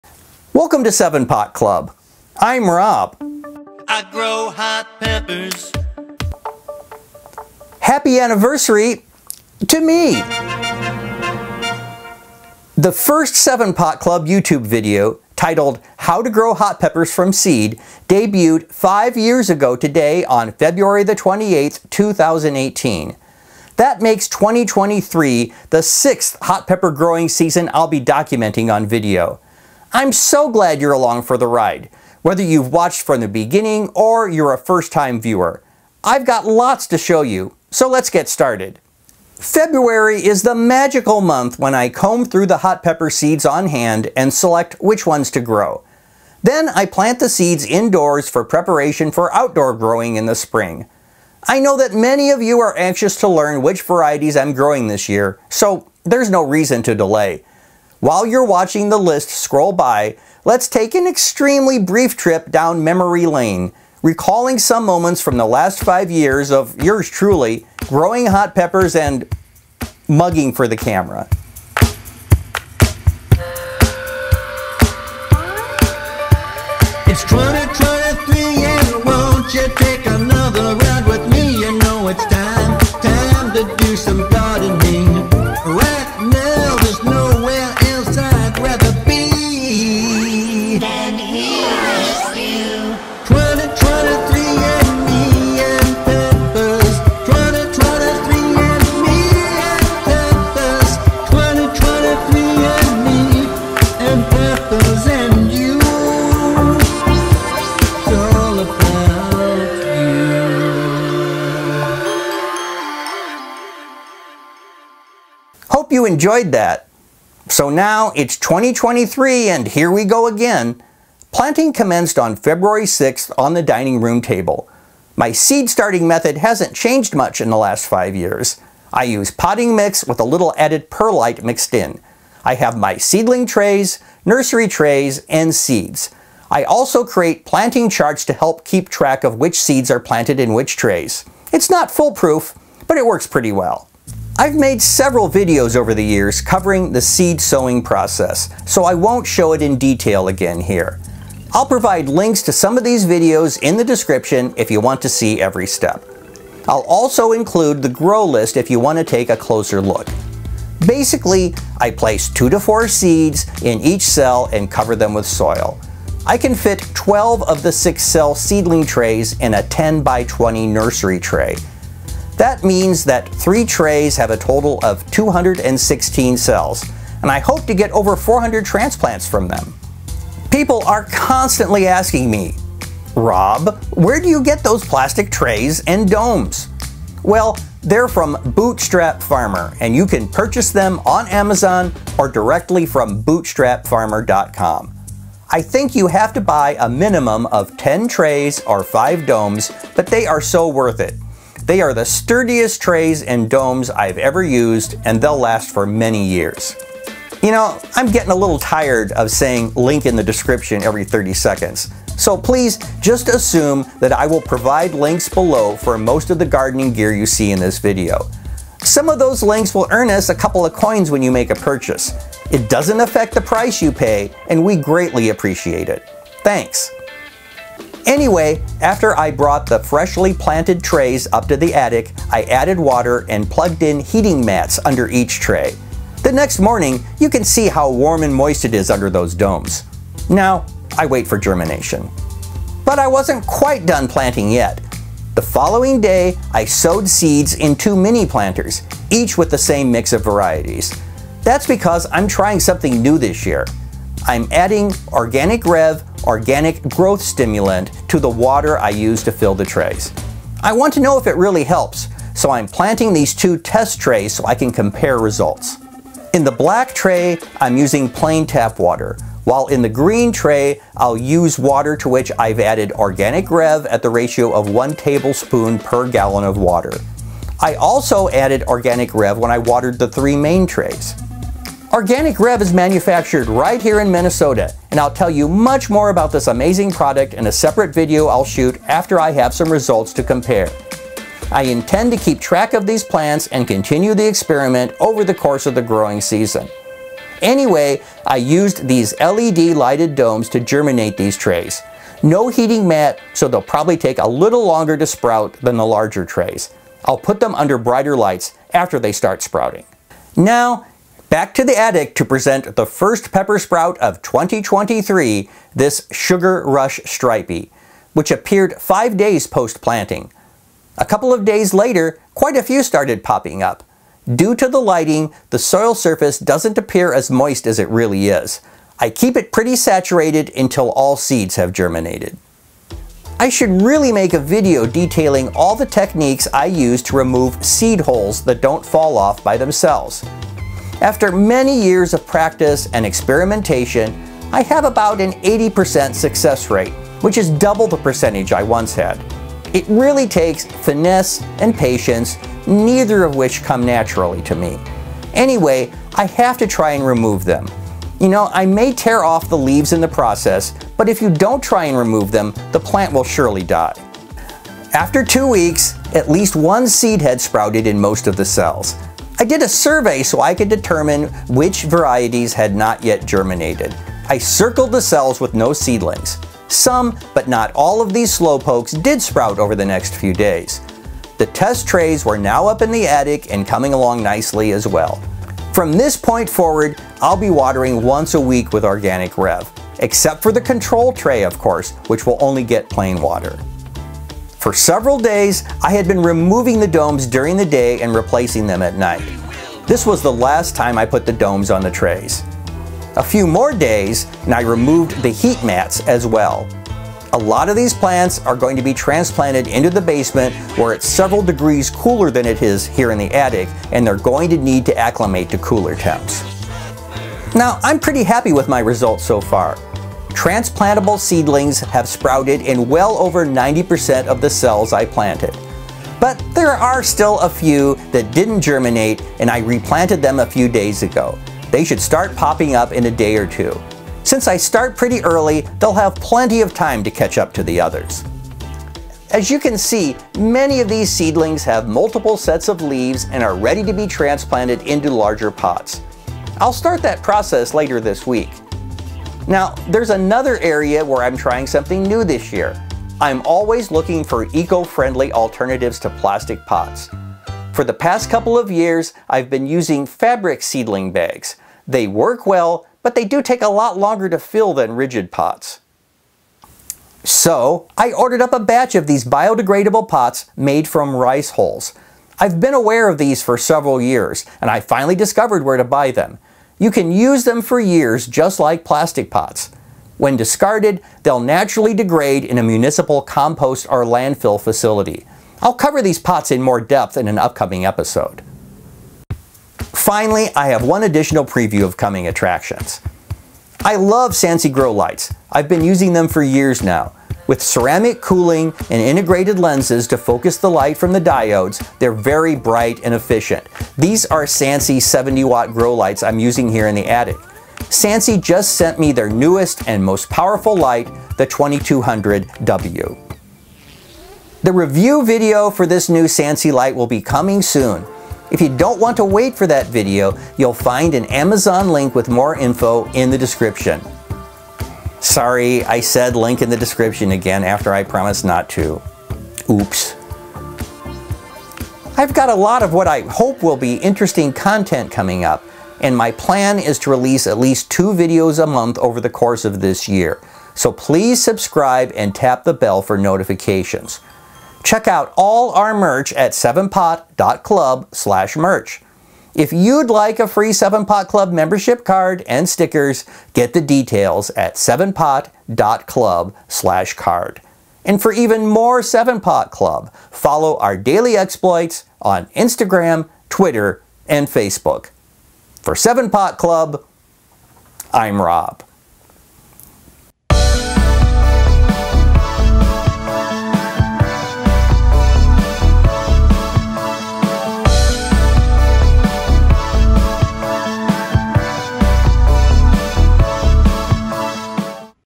2023 Hot Pepper Grow Season Kickoff
It’s 7 Pot Club’s 5th anniversary! In this episode: planting hot pepper seeds, Organic Rev test begins, where to find the best nursery trays and domes, removing seed hulls from pepper seedlings, biodegradable rice hull seedling pots, new SANSI 2200W grow light, and more.

2023 Grow List
7 Pot Barrackpore
7 Pot Brain Strain Red
7 Pot Brain Strain Yellow
7 Pot Chaguanas Red
7 Pot Jonah
7 Pot Orange-Yellow
7 Pot Primo
7 Pot Primo White
7 Pot SR Strain
Adrenalina
Adrenalina Port
Aji Charapita
Aji Charapita Iquitos
Aji Charapita Red
Aji Ethiopian Fire
Aji Pineapple
Aji Russian Yellow
Aji Strawberry Drop
Avenir
BBG Midnight Blue
Beast
Bhut Jolokia Solid Gold
Bhut Jolokia Strain I
Bingo Orange
Bingo Purple/Peach
Black Pearl X Blue Xmas X Mutant X Mattapeño F1
Blood Ghost
Blood Ghost J.S.
Blue Ghost Purple
Blue Xmas X Bleeding Raw White X Mutant Candlelight
Blush Reaper
Bolivian Rainbow
Brain Collapse Peach
Bugle A.A. Red F5
California Reaper
Candy Cane Chocolate Cherry
Candy Coat D.D.
Capsicum Galapagoense Long
Carl's Bananapeño
Carolina Reaper
Charapón Amarillo
Chiltepin Amarillo
Chocolate Massacre (Squatty)
Chupabrain Peach (Faddas)
Crazy Boy (Faddas)
Cumari Pollux
Da Hulk
Darth Maul (Faddas)
Dragon’s Breath
Dragons Breath Stinger
Dwarf Chiltepin
Ecuadorian Red Pepper For Hell
Farmer’s Market Jalapeño
Fox Dark Naga Red
Ghostly Jalapeño
Habanero Christina
Habanero Martinique
Habanero Vietnam
Hallow's Eve
Hawaiian X Ghost
Hot Paper Lantern
Jay's Ghost Scorpion White
Jay's Pink
Jay’s Dee’s Reaper F4
Kaos
KS Lemon Starrburst
Leopard Reaper
Lesya
Lesya Orange
Leviathan Gnarly Scorpion
Limón
Long Tail Scorpion Orange
Madre de Rios
Mattapeño F5
Mattapeño X Mutant F2
Mattapeño X RH Lilac F2
MonsterGum Leopard
Moruga Blackjack
Mustard Tiger
Mutant Candlelight
Mutant Mix
Mutantgum
Naga Tiger D.D.
Neyde Rose Bullet (Faddas)
Olho de Mutum
Orange Scorpion
PC-1
Peach Reaper DS
Peach X DD
Pimenta Diomar
Pimenta Lisa
Pimenta Project No. 5 Purple/Orange F4
Pink Moruga Multicolor
Primotalii
Puma
Purira
Purple Gator Jigsaw
Purple Ghost Scorpion Peach
Purple Murupi
Reaper Pink Peach
Reaper Sbagliato
Reaper X Moruga
Red Ripple
Red Savina
Red Shark
Rhino DD Orange
Rhino DD Red
Rob’s Folly 2020
Rob’s Folly 2022
RXM SLP
Sbagliato Orange Reaper
Scotch Bonnet MOA
Scotch Bonnet Trenton Farmer’s Mkt
Secret Potion Chocolate Red
Sugar Rush Stripey
T-Rex Chocolate
T-Rex Mustard
T-Rex Yellow
Tangerine Tiger F4
Texas Rice
Thanos
The Beast Red
The Dream Purple/Peach
Tiger Jalapeno
Titanio
Trinidad Scorpion Butch T
Trinidad Scorpion Long SR
Trinidad Scorpion Moruga Caramel
Trinidad Scorpion Moruga Red
Xion Mutant
Zébrange

00:00 Intro
01:12 Grow list and song
01:56 Planting Day
03:00 Bootstrap Farmer Nursery Trays and Domes
04:02 Rev Organic Growth Stimulant
05:15 Peppers start sprouting
05:35 Removing seed hulls from seedlings
06:08 Germination survey and replanting
07:34 Biodegradable Rice Hull Seed Pots
08:09 SANSI grow light 2200W
08:51 Outro